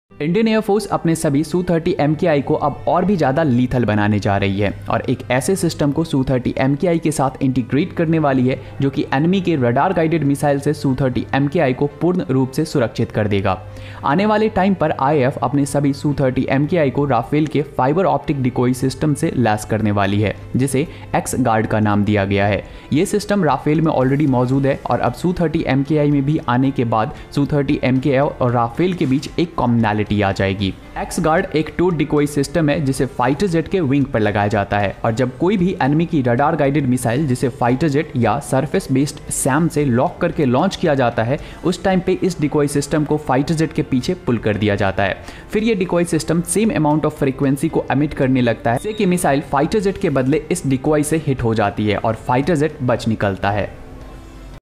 Su 30 mki x-gaurd decoy
Decoy means a thing that intends to attract someone by deceiving them. So basically Decoys on fighter aircraft work in a way, that they will attract the incoming threat and deceive it towards themselves. And here the term “towed” means “to pull someone/something behind one object. So, basically towed decoys are such that they are pulled along with the aircraft and that decoy will be part of the EW suits of aircraft.

This countermeasure basically for the RF-guided missiles i.e. Radar Guided missiles.  It works together with the aircraft’s electronic warfare system to provide radar jamming. In addition, it can also be used in a backup mode as a signal repeater, which allows it to lure incoming missiles away from their actual target.